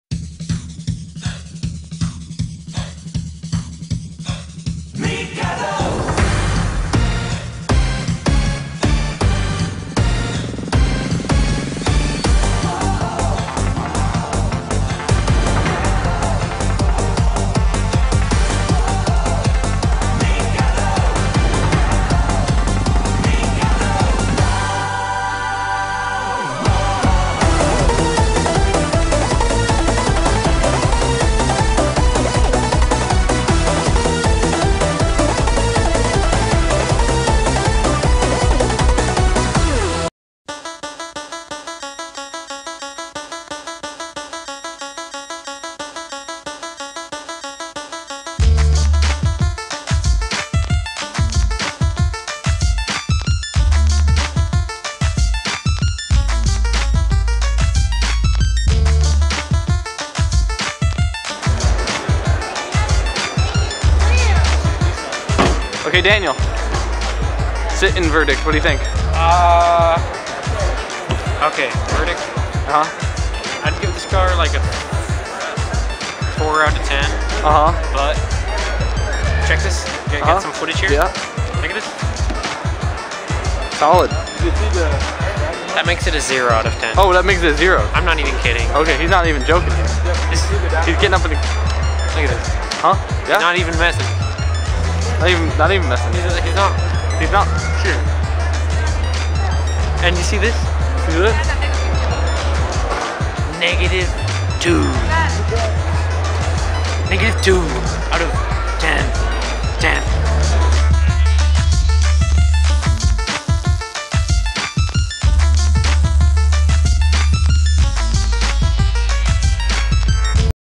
Daniel Reviews: Mini Cooper S
at the sf auto show we found this sad sack